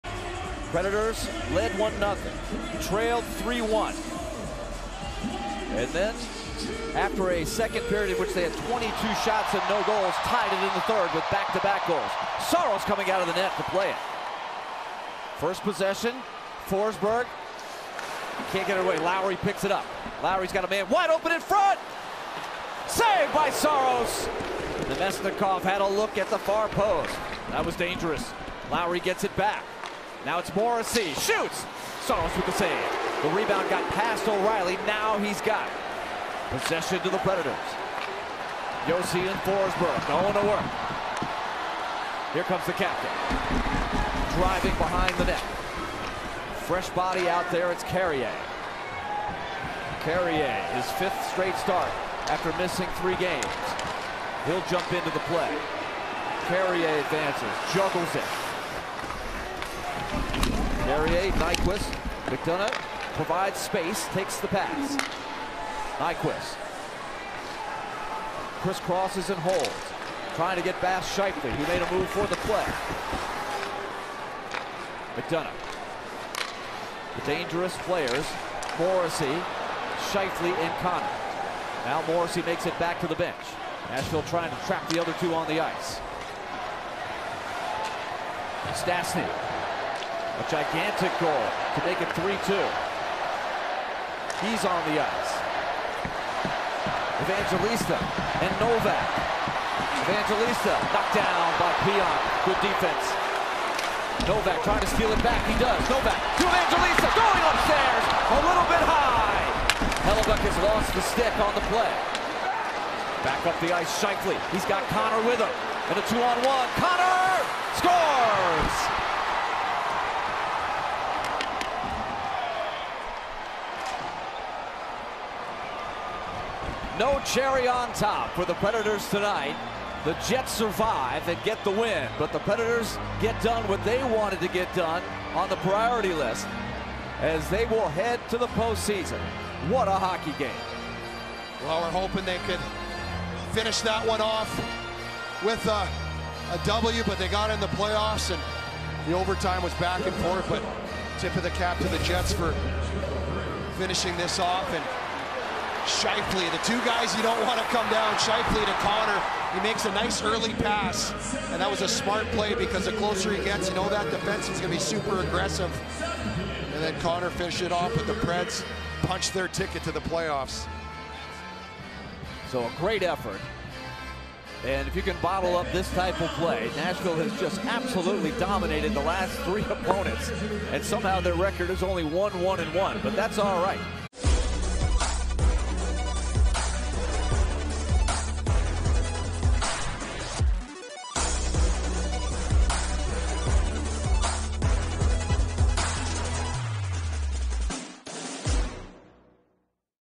Winnipeg Jets at Nashville Predators | FULL Overtime Highlights - April 9, 2024
Watch the full overtime between the Winnipeg Jets and the Nashville Predators from April 9, 2024.